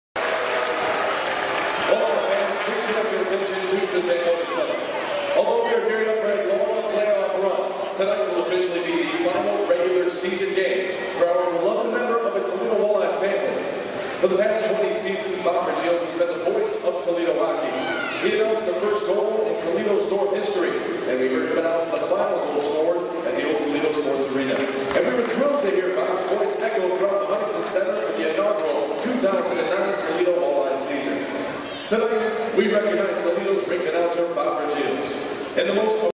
Bobb's presentation
Huntington Center
3/24/13
Bobb Vergiels last regular season game at Huntington Center